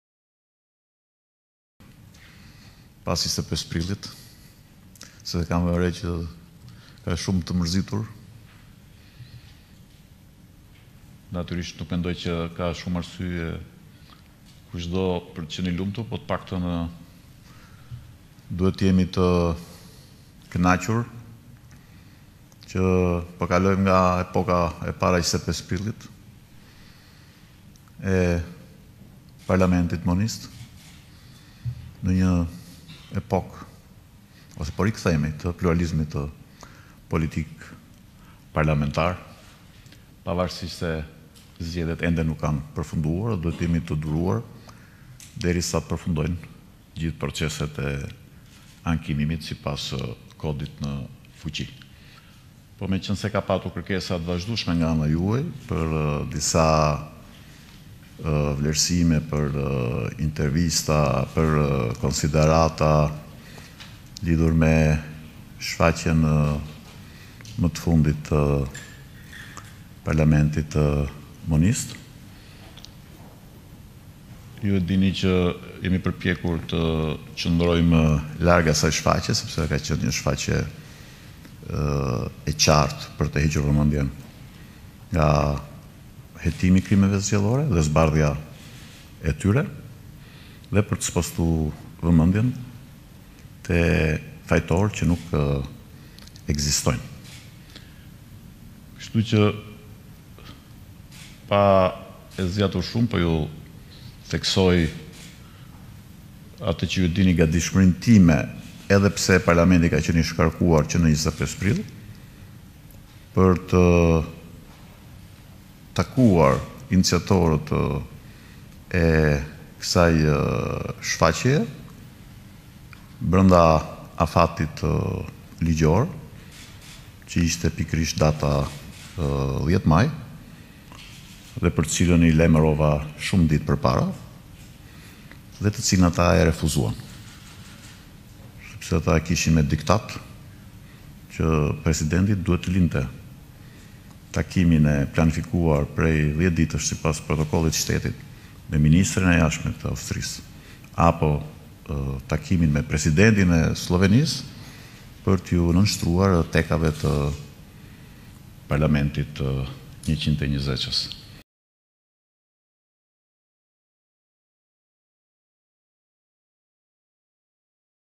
Meta: Tentativë e parlamentit monist për të tërhequr vëmendjen nga krimi zgjedhor
TIRANË- Presidenti i Republikës, Ilir Meta deklaroi sot se nuk është një shqetësim për të vendimi i Kuvendit për ta shkarkuar. Meta e cilësoi vendimin një shfaqje e parlamentit monist, ndërsa shtoi se Kuvendi kërkon të heqë vëmendjen nga hetimi dhe zbardhja e krimeve zgjedhore.

‘’Pas 25 prillit se e kam vënë re që jeni të mërzitur, natyrisht ka shumë arsye për çdo për të qenë i lumtur, duhet të jemi të kënaqur që po kalojmë nga epoka e para 25 prillit, e parlamentit monist, në një epokë ose po rikthehemi në pluralizmin politik parlamentar, pavarësisht se zgjedhjet ende s’kanë përfunduar, duhet të durojmë, derisa të përfundojnë proceset e ankimimit.